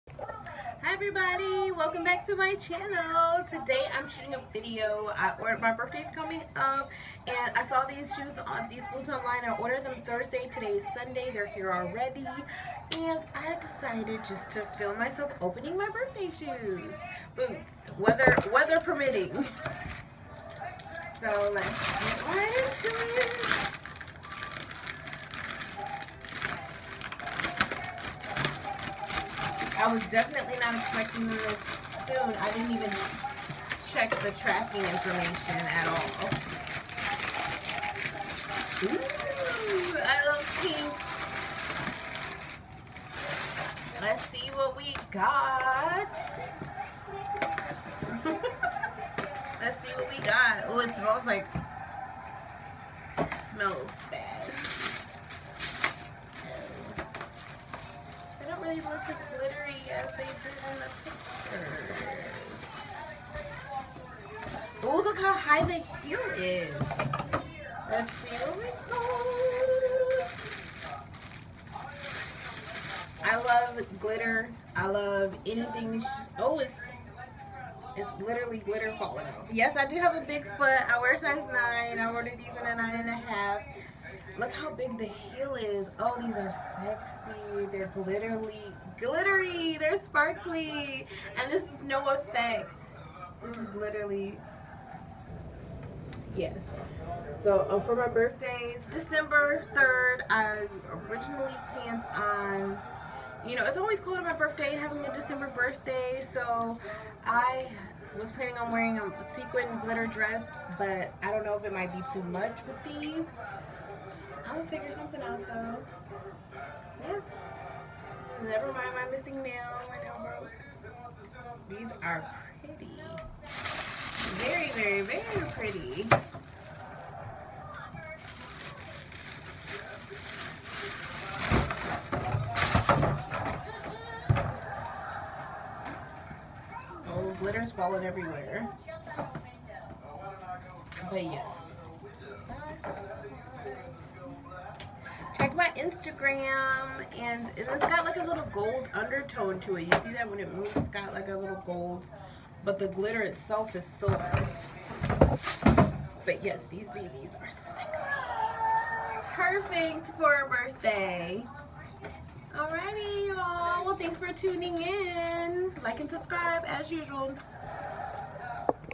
ShoeDazzle Unboxing 📦 | Birthday shoes 🎁
Unboxing boots I ordered from ShoeDazzle for my birthday.